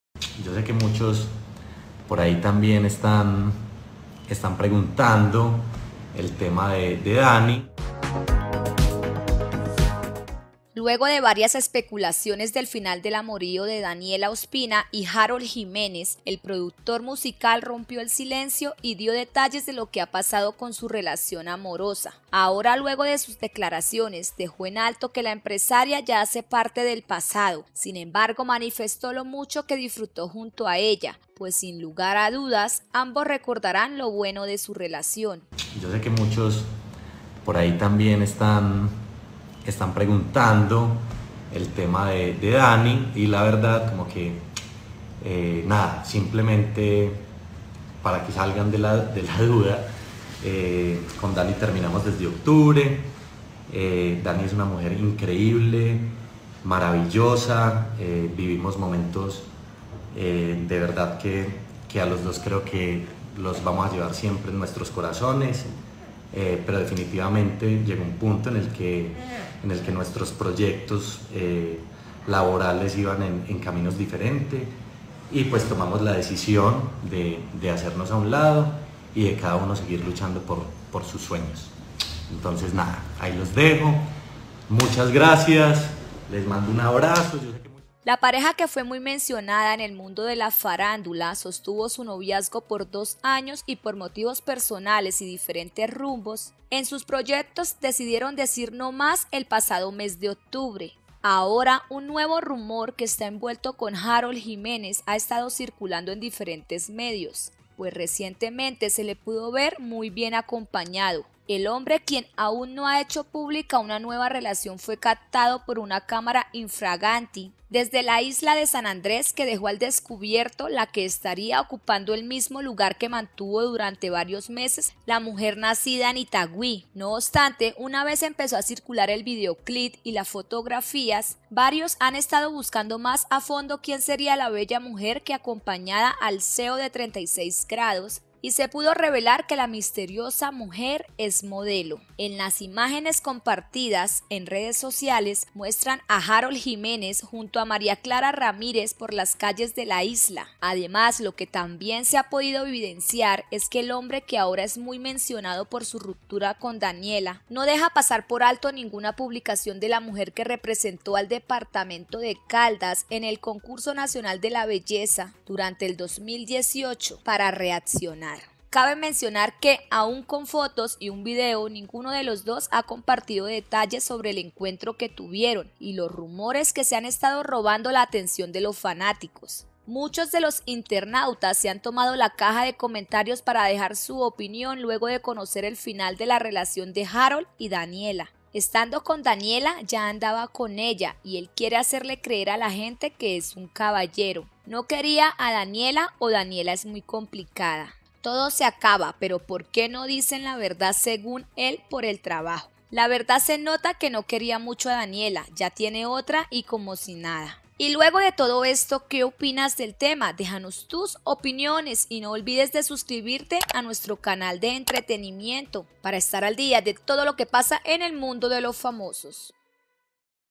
Por esta razón: SE CONOCE el motivo por el que Harold Jimenez y Daniela Ospina DIJERON NO MÁS
🔥 Un nuevo video TODOS LOS DÍAS